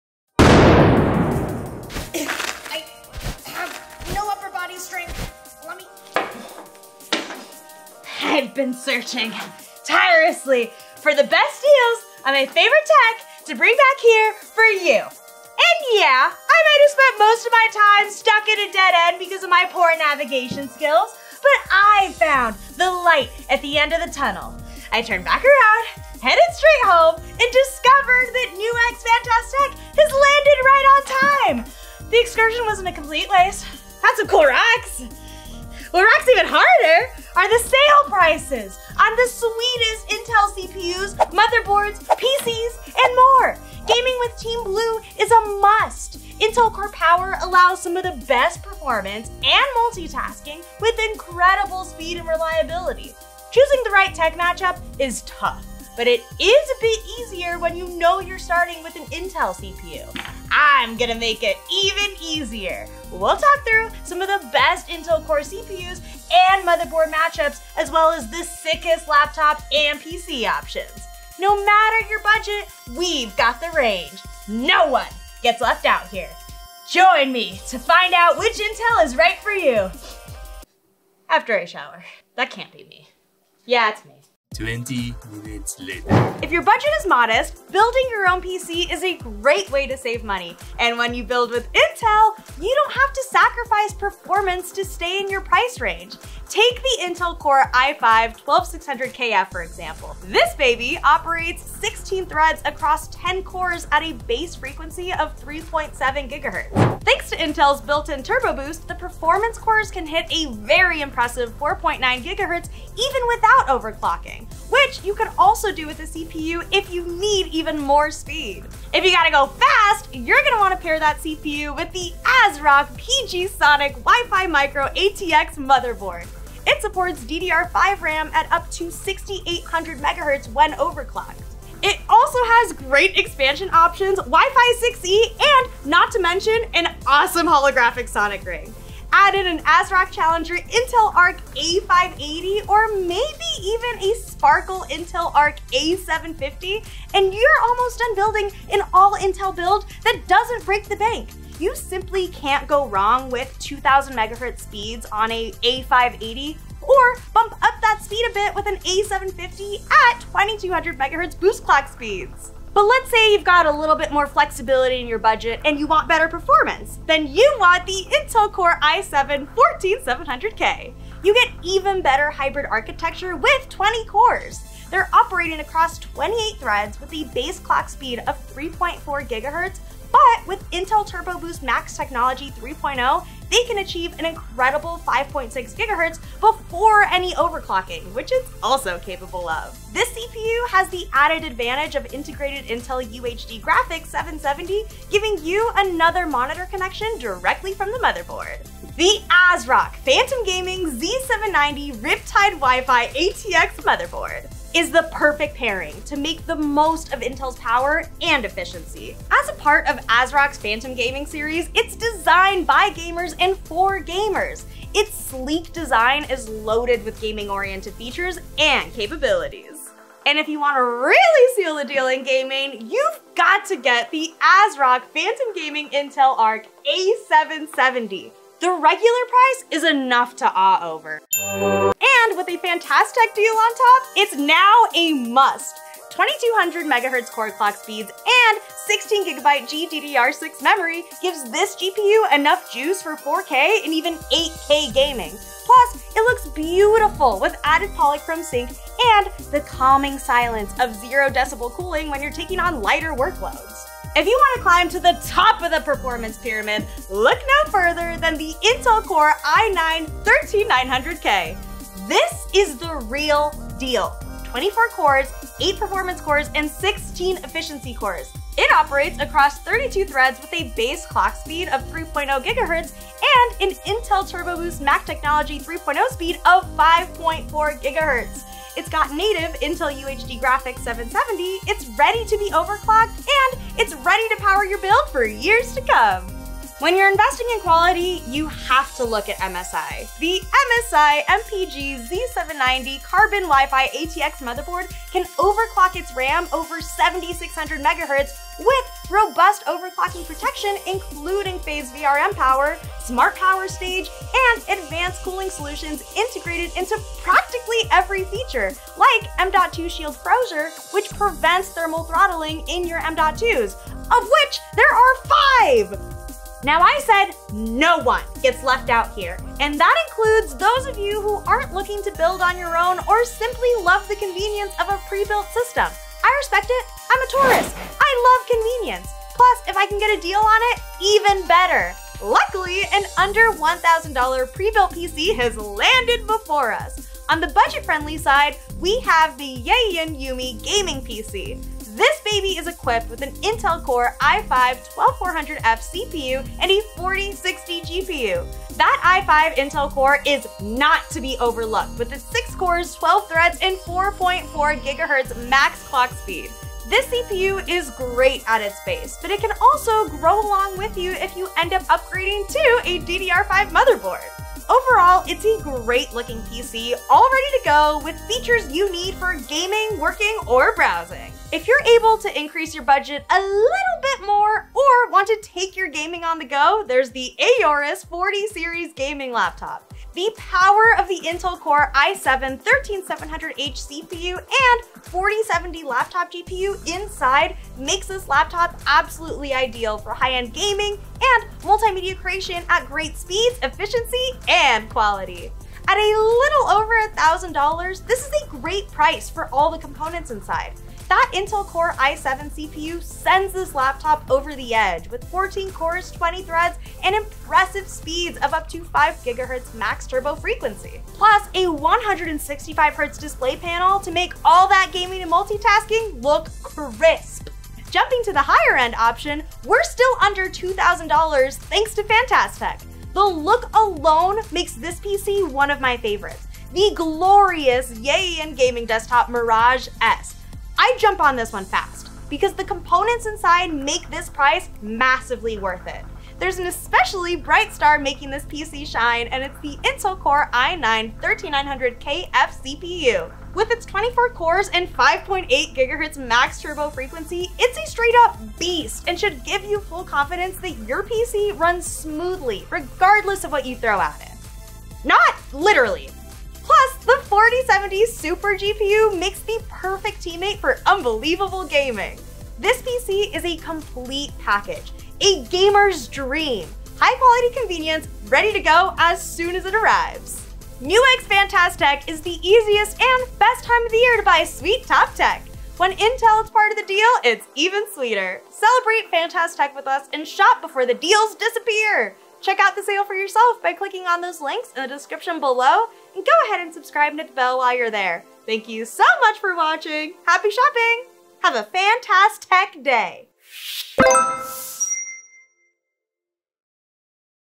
Fantastech Guide To The BEST of Intel
Fantastech is HERE and Tori is your guide to explore the BEST tech from Intel! Fantastech is bringing top deals whether you’re seeking components or a complete gaming PC or Laptop. From 12th to 14th Gen Intel Core CPUs, we’ll explore the right match for you! The deals won’t last forever, so join us and build TODAY! Let us know which tech you’re most excited about!